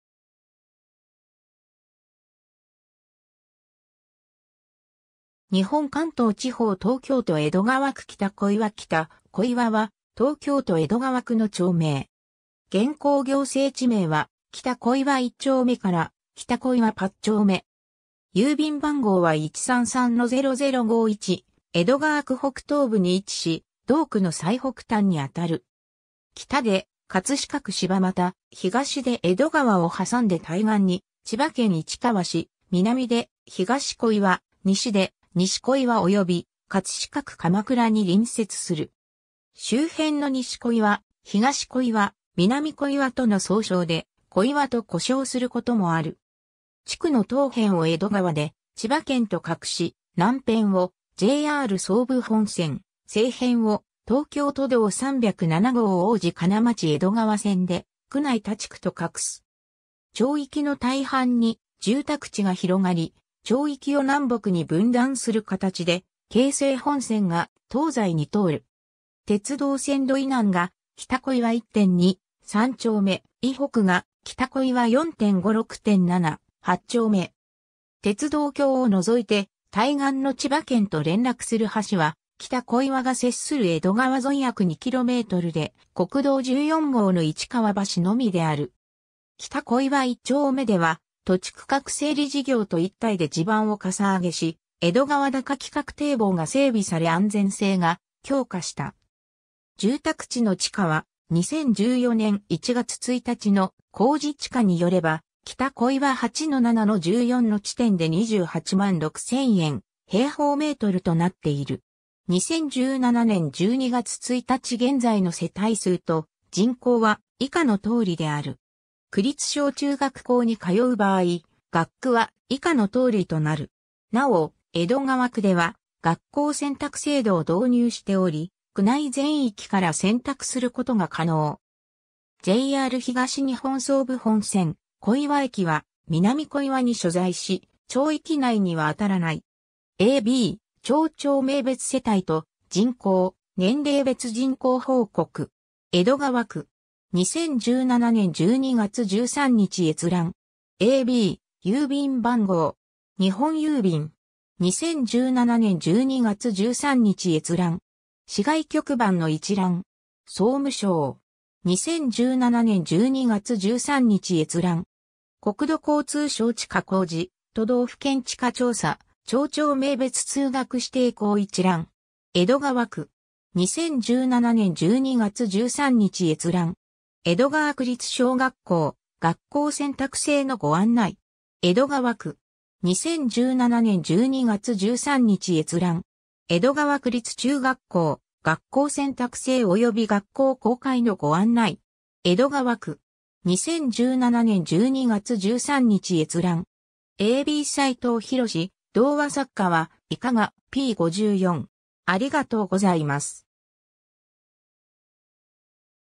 北小岩
日本 」 関東地方 」 東京都 」 江戸川区 」 北小岩 北小岩（きたこいわ）は、東京都江戸川区の町名。
現行行政地名は北小岩一丁目から北小岩八丁目。
郵便番号は133-0051 江戸川区北東部に位置し、同区の最北端にあたる。
北で葛飾区柴又、東で江戸川を挟んで対岸に千葉県市川市、南で東小岩、西で西小岩および葛飾区鎌倉に隣接する。
周辺の西小岩、東小岩、南小岩との総称で「小岩」と呼称することもある。
地区の東辺を江戸川で千葉県と画し、南辺をJR総武本線、西辺を東京都道307号王子金町江戸川線で区内他地区と画す。
 町域の大半に住宅地が広がり、町域を南北に分断する形で京成本線が東西に通る。
鉄道線路以南が北小岩一・二・三丁目、以北が北小岩四・五・六・七・八丁目。
鉄道橋を除いて対岸の千葉県と連絡する橋は北小岩が接する江戸川沿い約2kmで国道14号の市川橋のみである。
 北小岩一丁目では土地区画整理事業と一体で地盤を嵩上げし、江戸川高規格堤防が整備され安全性が強化した。
 住宅地の地価は、2014年（平成26年）1月1日の公示地価によれば、北小岩8-7-14の地点で28万6000円/m2となっている。
 2017年（平成29年）12月1日現在の世帯数と人口は以下の通りである。
なお、江戸川区では学校選択制度を導入しており、区内全域から選択することが可能。
 JR東日本総武本線・小岩駅は南小岩に所在し、町域内にはあたらない。
 ^ a b “町丁目別世帯と人口・年齢別人口報告”. 江戸川区 (2017年12月8日). 2017年12月13日閲覧。
 ^ 国土交通省地価公示・都道府県地価調査 ^ “町丁目別通学指定校一覧”. 江戸川区. 2017年12月13日閲覧。
 ^ “江戸川区立小学校「学校選択制」のご案内”. 江戸川区 (2017年5月12日). 2017年12月13日閲覧。
 ^ “江戸川区立中学校「学校選択制」及び学校公開のご案内”. 江戸川区 (2017年7月12日). 2017年12月13日閲覧。
 ^ a b 斉藤洋『童話作家はいかが』（講談社）p.54